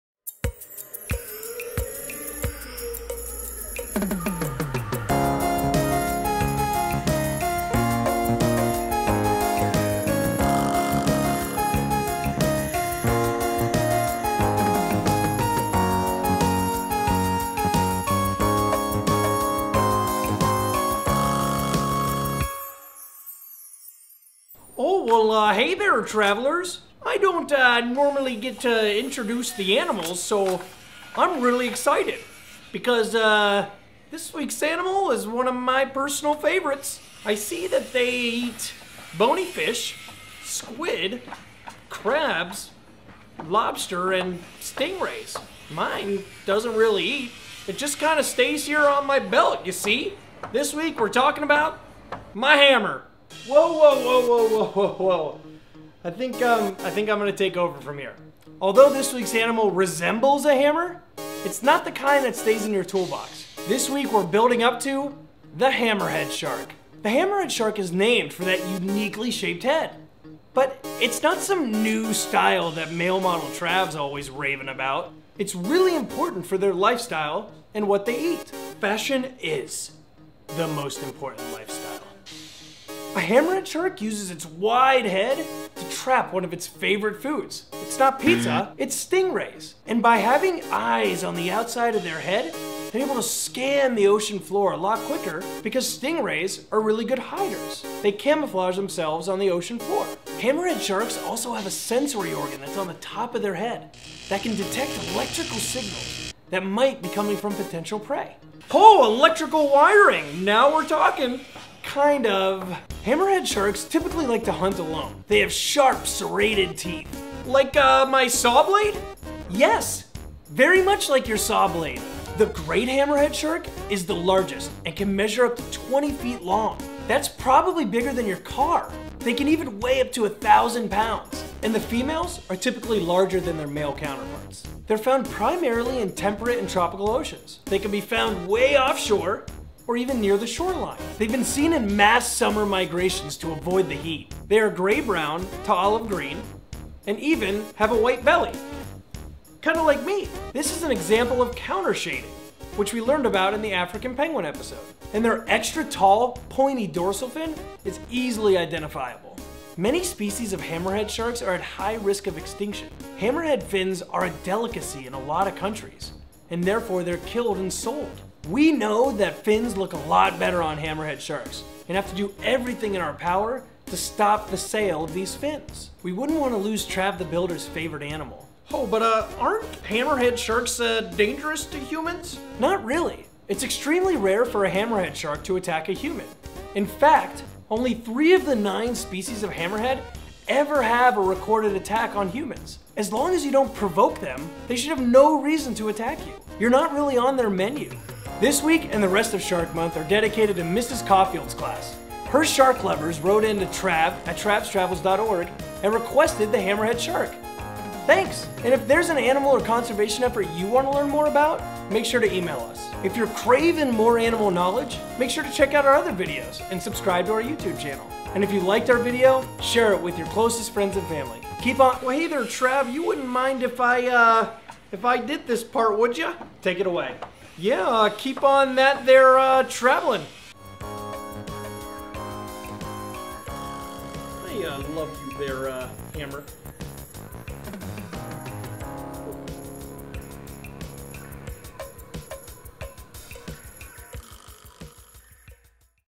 Hammerhead Shark
HAPPY SHARK MONTH TRAVelers!  This month we're all about sharks as we countdown to Discovery Channel's annual Shark Week! Week one is HAMMER TIME! Watch TRAV try and teach Trav the Builder the differences between construction tools and the Hammerhead Shark. Spoiler alert - hammers are not the same as Hammerheads!

Do you have any Shark Month questions? Ask Trav all month long and get your questions answered online!

To learn more about the Hammerhead Shark, check out travstravels.org

The Travs Travels team is -
Travis Carter
Asher Coffield
Michael Favata
Jason Homer
Michael Schondek